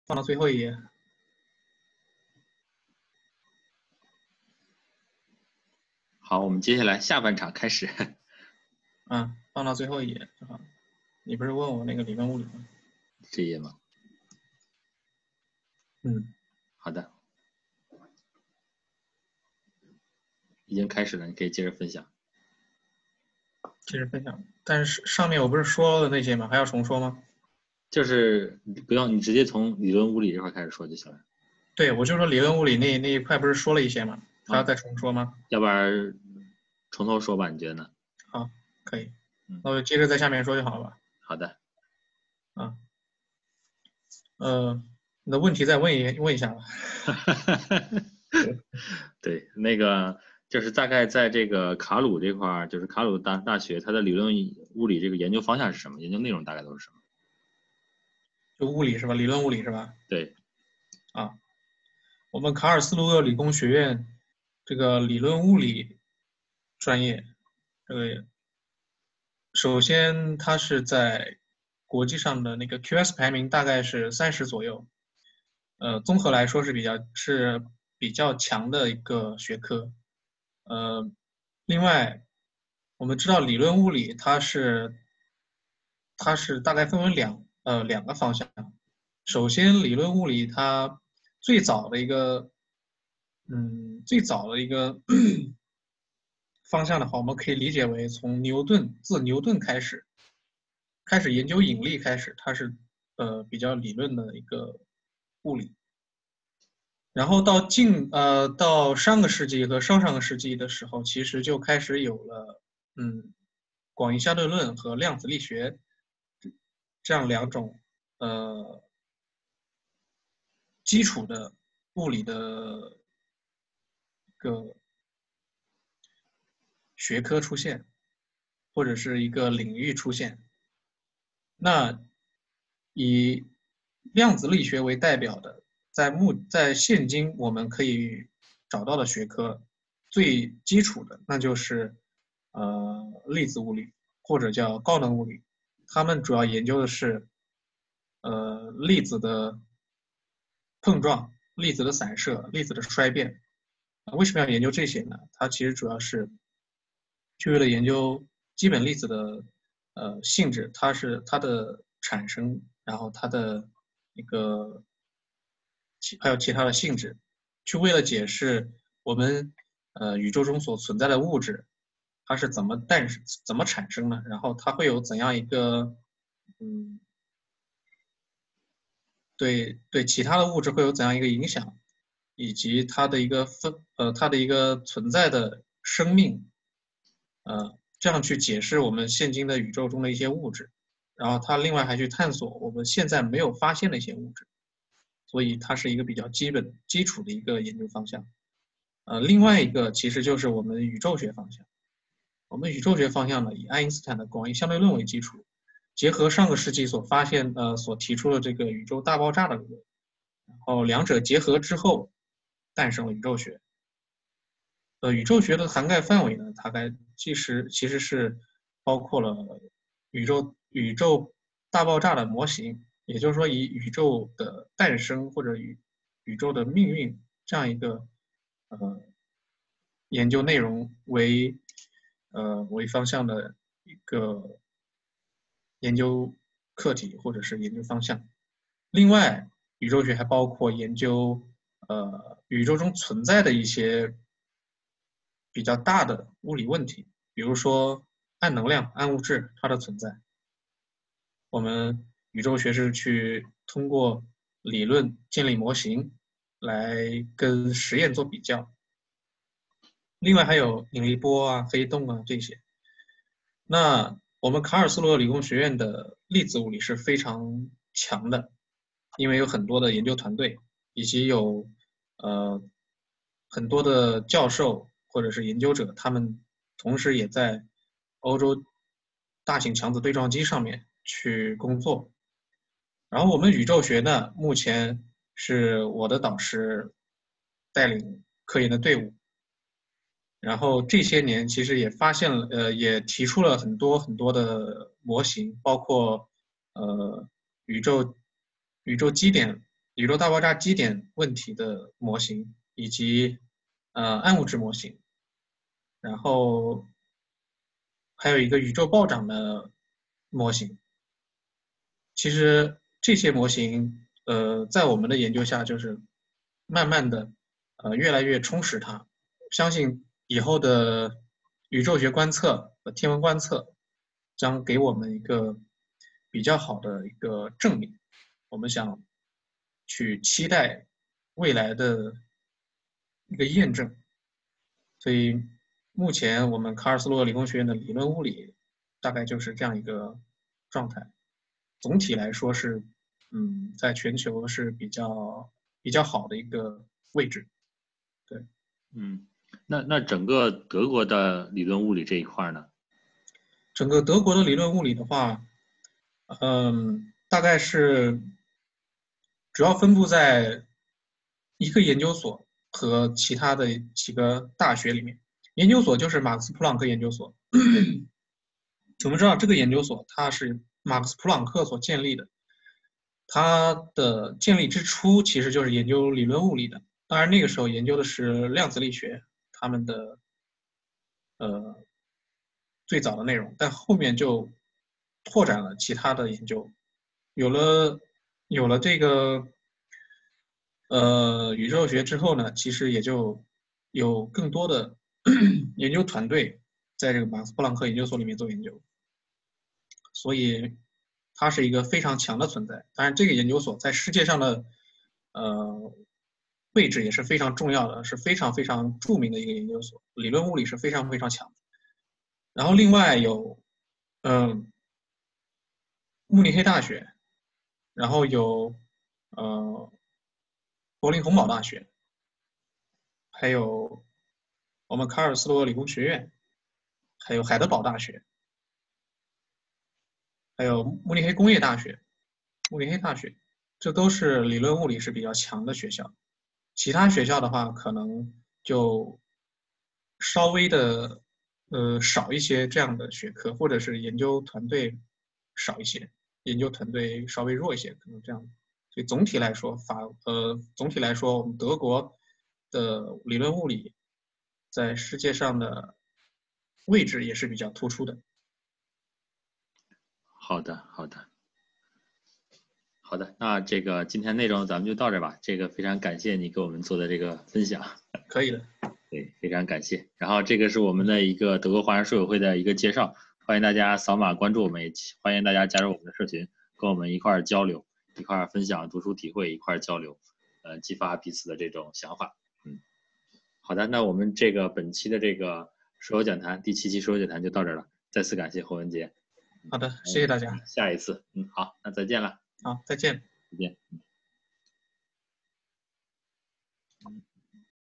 [书友讲坛] 第七期 The Universe（下）| 卡尔斯鲁厄理工大学及其他德国大学的理论物理（宇宙学方向）研究现状简介
主讲人：Karlsruher Institut für Technologie (KIT) 理论物理 宇宙学方向 Ph.D. Wenjie Hou 侯文杰
内容：KIT 以及德国理论物理研究概况